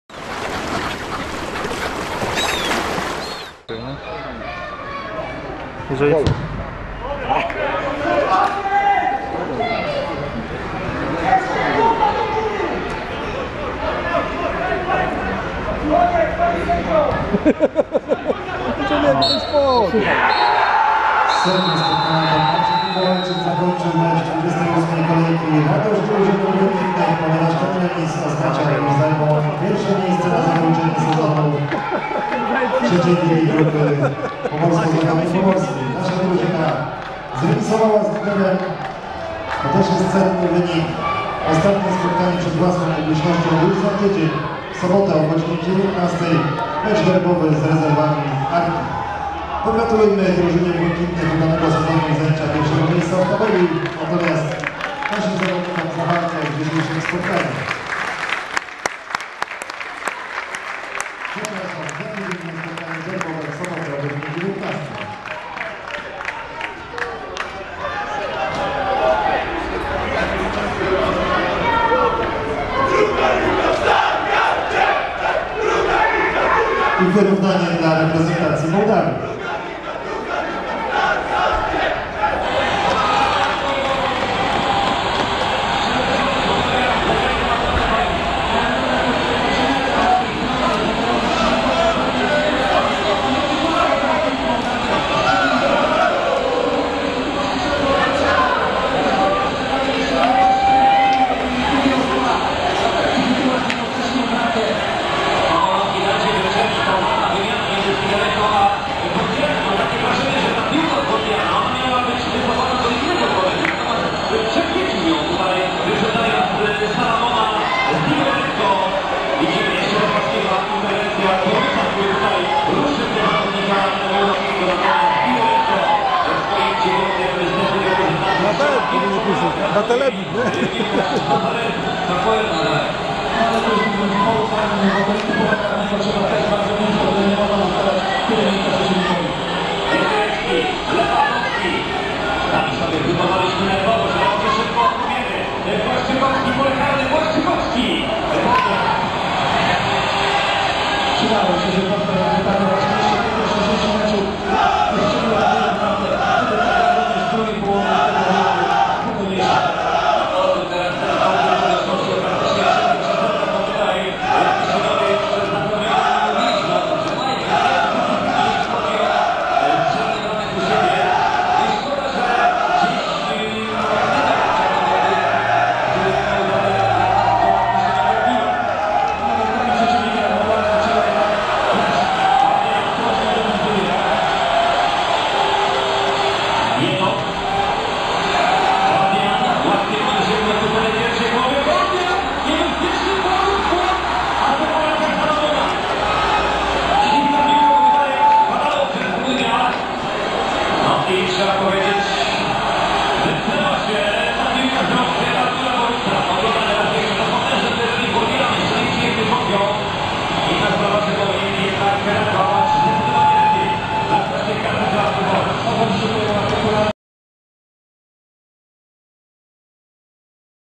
Bałtyk Gdynia - Błękitni Stargard Szczeciński 1:1 /7.06.2013/ radość
III liga - 28. kolejka
22' Krzysztof Bułka